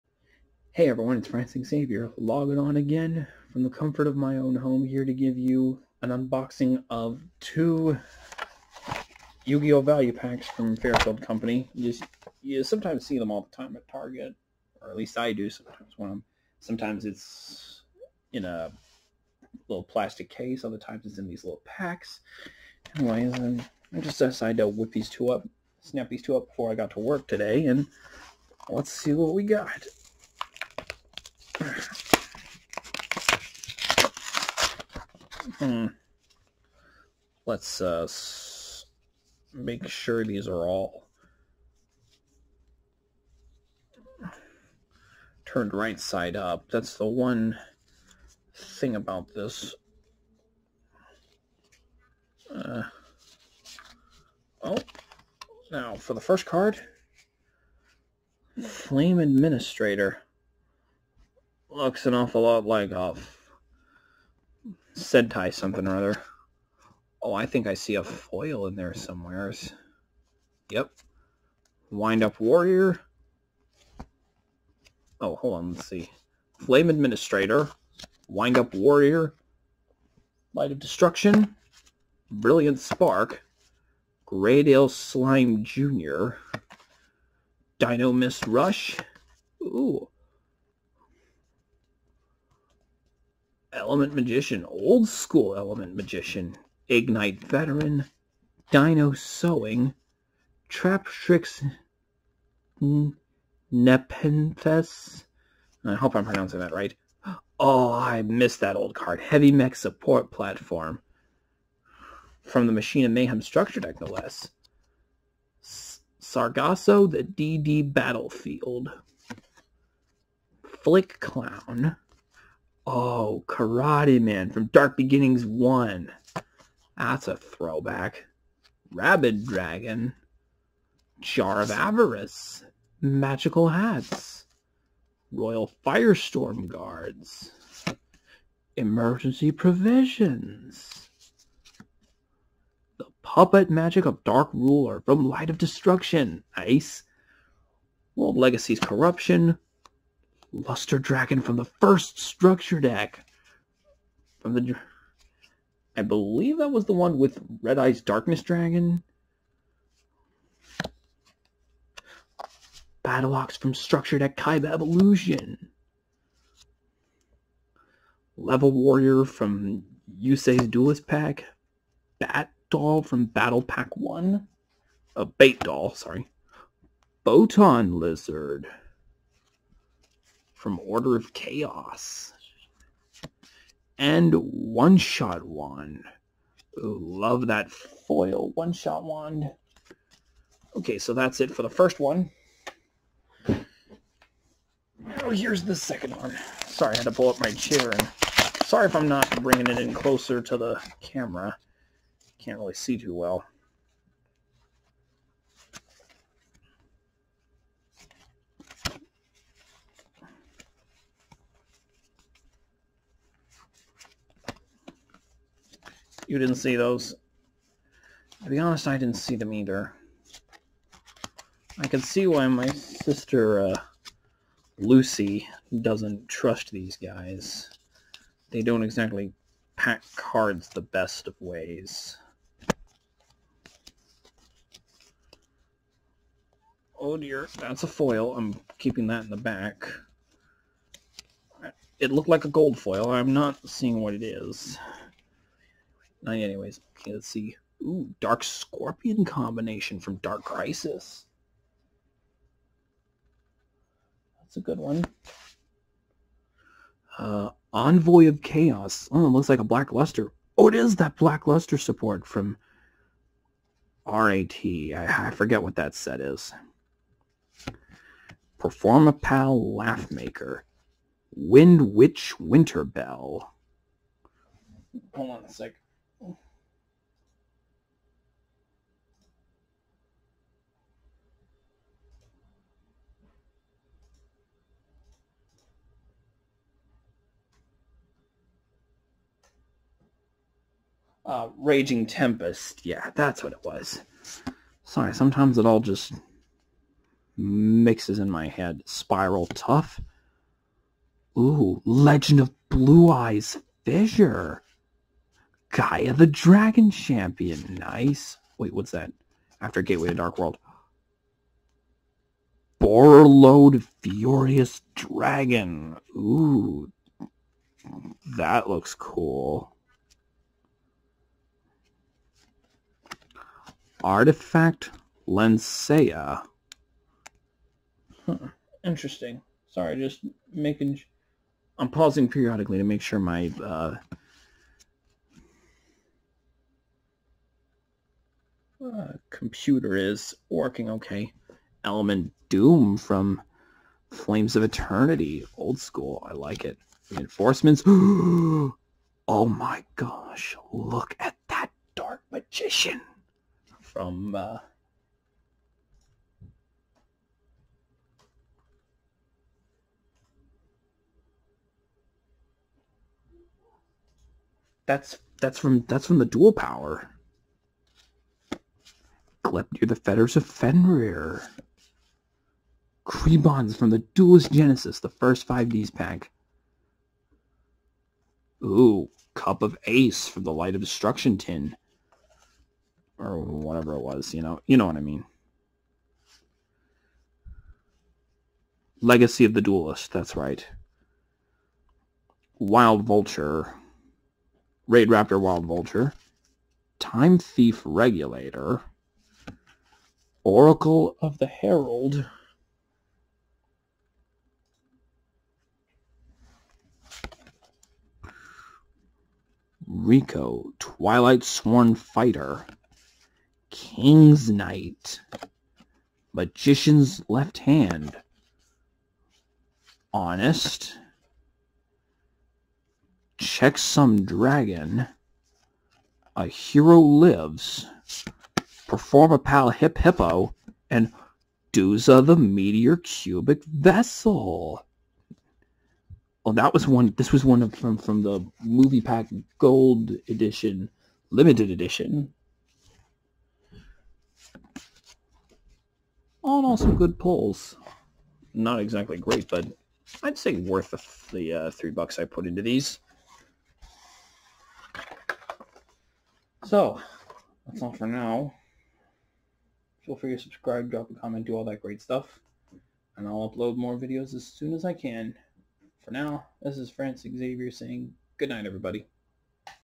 Francis Unboxes Yu-Gi-Oh! Fairfield Value Packs
I recorded this almost a week ago, but I never had the chance to upload this because of work, but here it is! These packs don't have much and they're haphazardly put in there, but it's interesting to see what you can find in these.

-Francis Xavyer logging off!